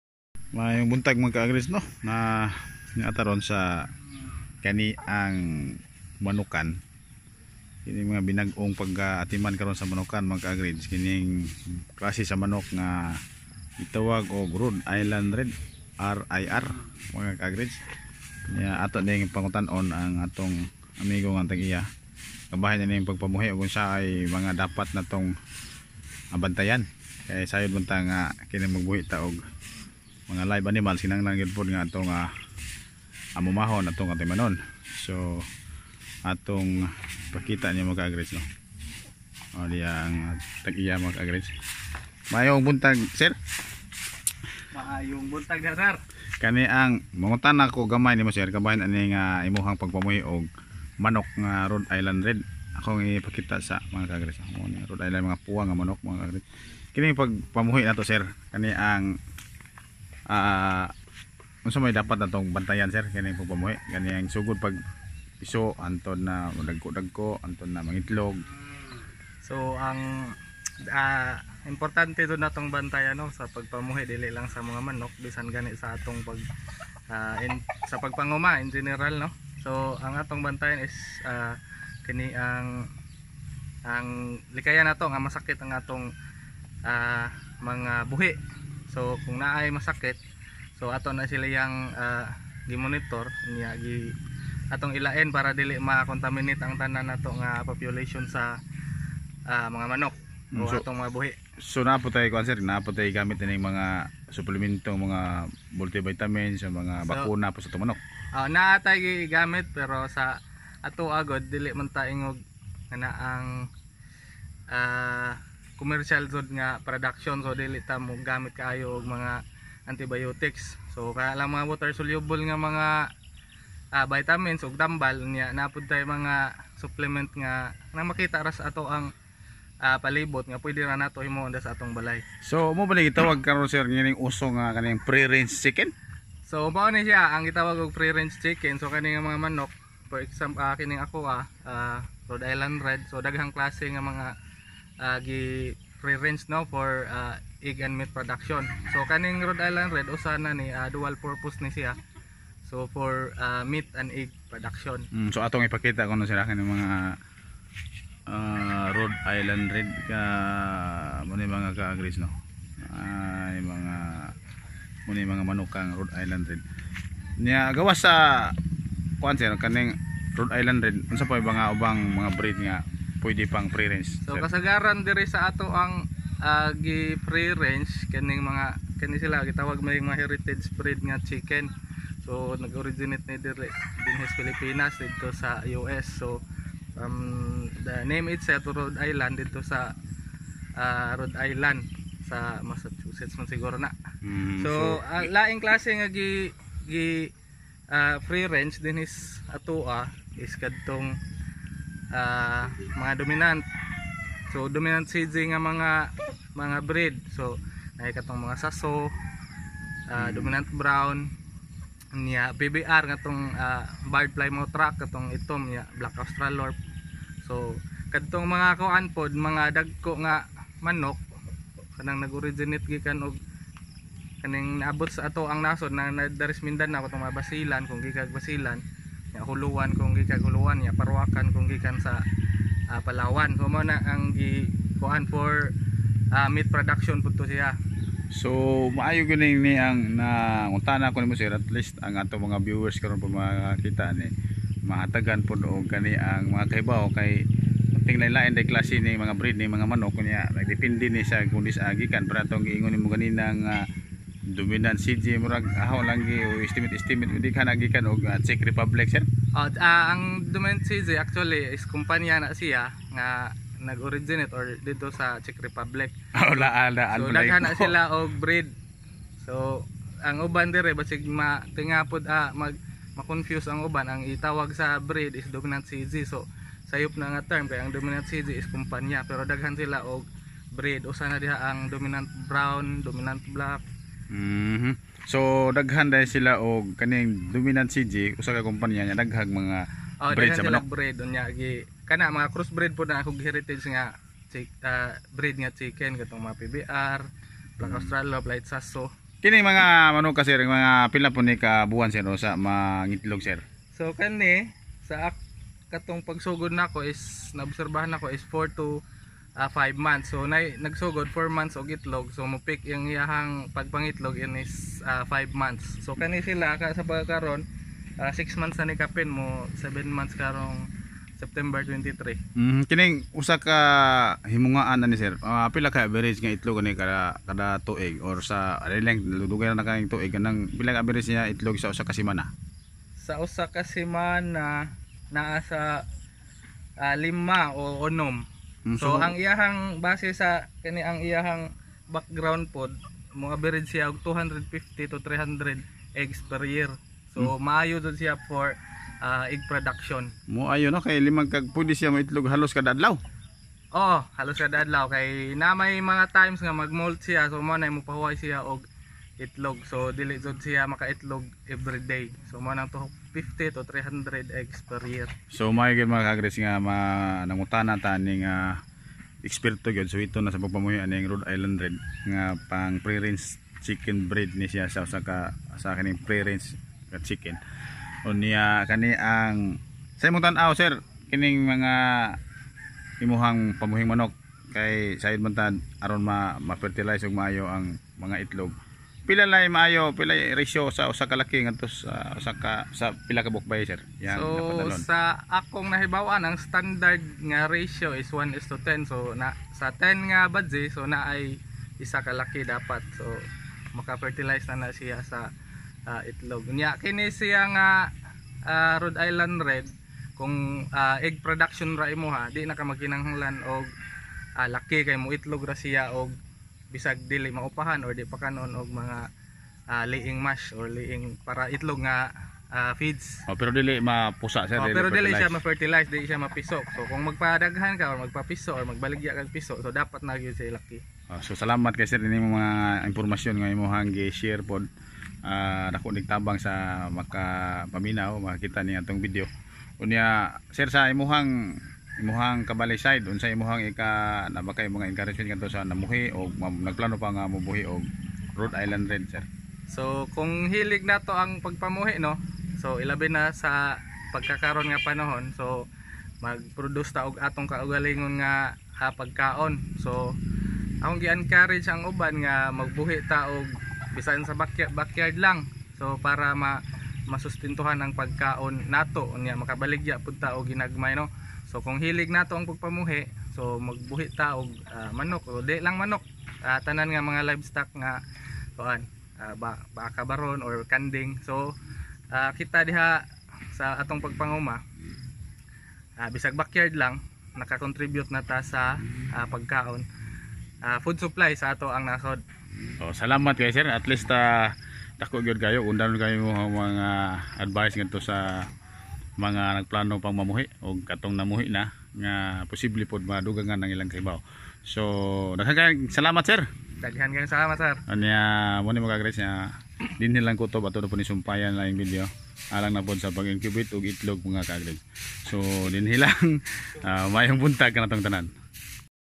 Pagpadagahan sa manok nga Rhod Island Red (RIR) with sir Sunday
Ang Pagpamuhi ug taas nga klase sa manok dili lang makatabang sa adlaw adlaw nga panginahanglan mahakahatag sab kini ug dugang kita para sa pamilya ug supply sa pagkaon sa atong lokalidad.